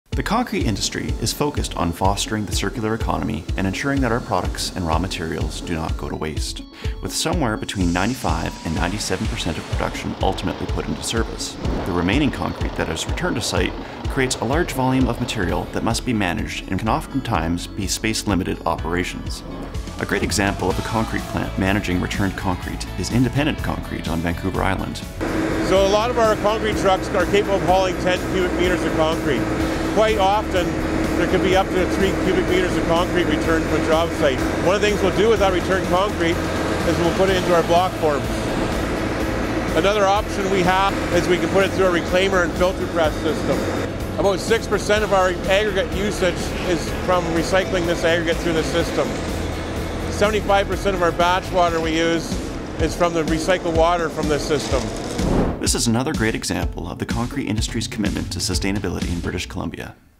Recycled Concrete
Concrete trucks require access to water for various purposes, including washing out the drum and chute after a pour to avoid the build-up and hardening of concrete.

To minimize water consumption, industry professionals have adopted best practices, such as using high pressure hoses to ensure effective cleaning of the drum and chute and using a bucket to capture runoff water and dispose of it properly.

These best practices are just some of the ways that the concrete industry in BC is striving to minimize its impact on the environment and protect our natural resources.